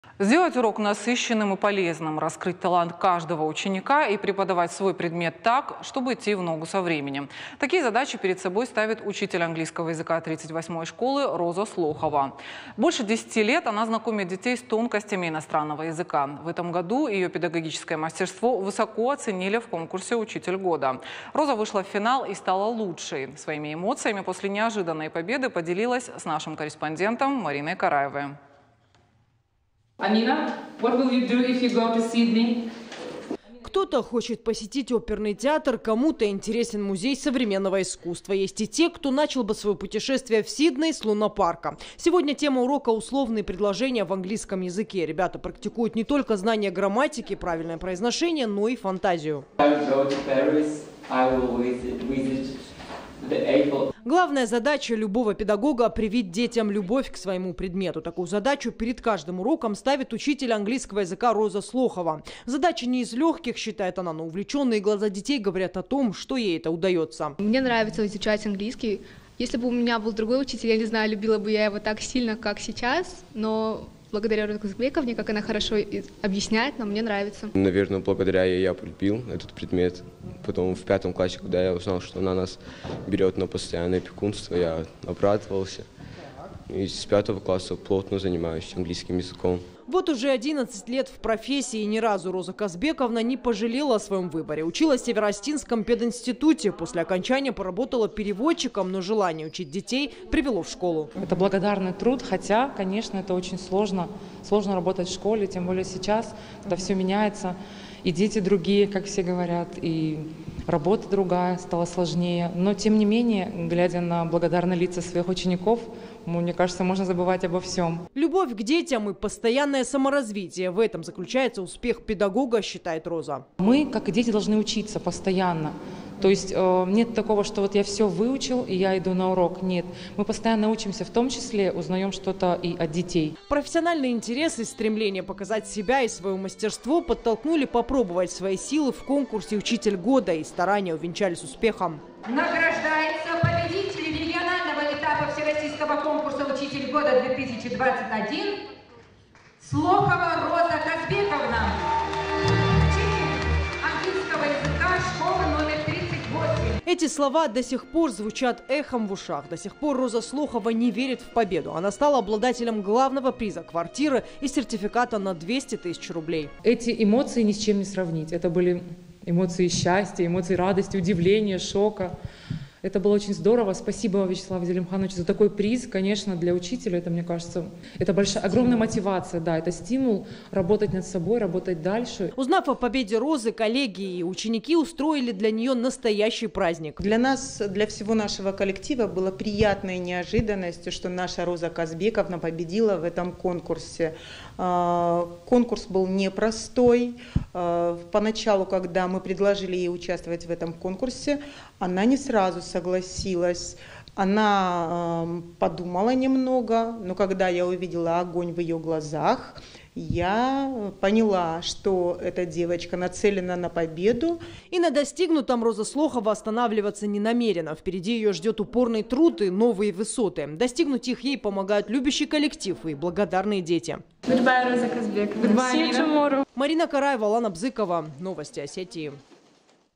Преподаватель английского Роза Слохова стала Учителем Года
Сделать урок насыщенным и полезным. Раскрыть талант каждого ученика и преподавать свой предмет так, чтобы идти в ногу со временем. Такие задачи перед собой ставит учитель английского языка 38 школы Роза Слохова. Больше 10 лет она знакомит детей с тонкостями иностранного языка. В этом году ее педагогическое мастерство высоко оценили в конкурсе “Учитель года”. Роза вышла в финал и стала лучшей.
Мы рядом в:
ok.ru/irystontv